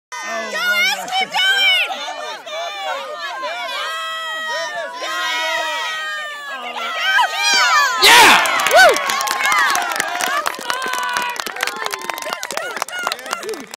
Ezra Hapner - U8 soccer SHARKS
8:30 AM game, 4th Quarter. game tying goal.